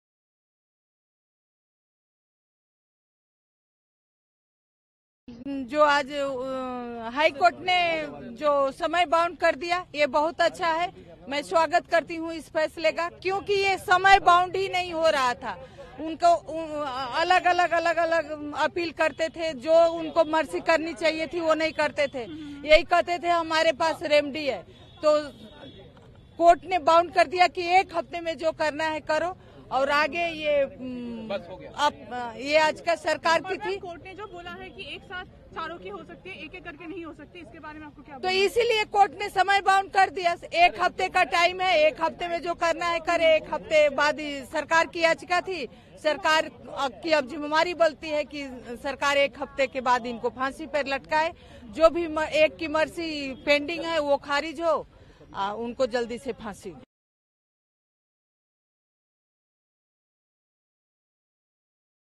Nirbhaya case: All convicts will be hanged together, says Delhi HC
Nirbhaya's Mother Asha Devi, welcomed the Delhi High Court's decision to dismiss the Centre's plea challenging trial court order which had stayed the execution of all 4 convicts. Court said death warrant against all 4 convicts can't be executed separately. "I welcome Delhi High Court's verdict.